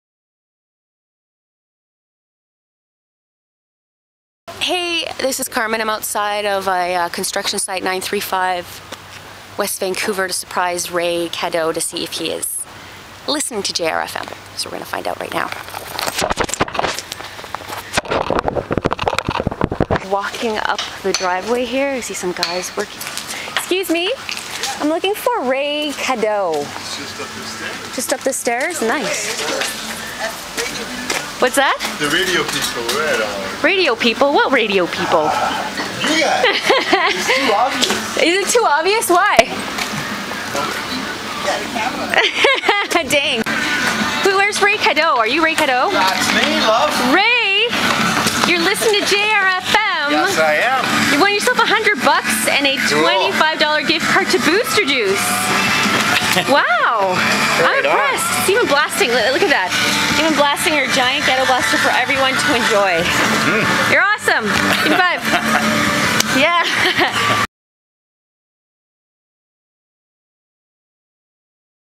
Listen at Work with Carmen Cruz
Carmen Cruz heads to a job site in North Vancouver!